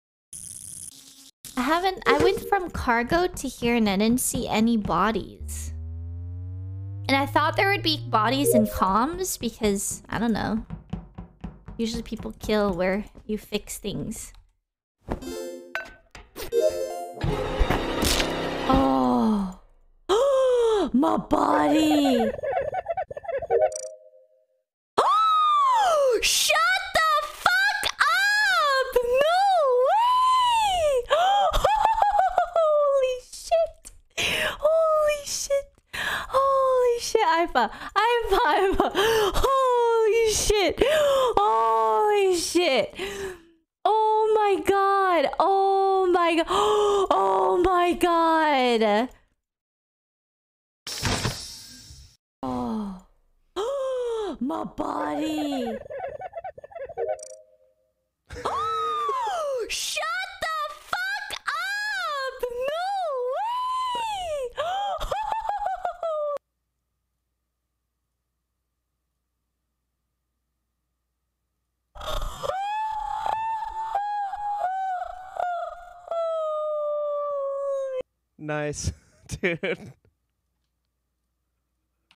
Janet's Body Joins @Dakotaz's Collection
Janet's Body Joins Dakotaz's Collection + Steve's Reaction

0:00 Janet's POV
0:52 Steve's Reaction

Janet's Timestamped VOD: - 01:09:57
Steve's Timestamped VOD: - 01:48:51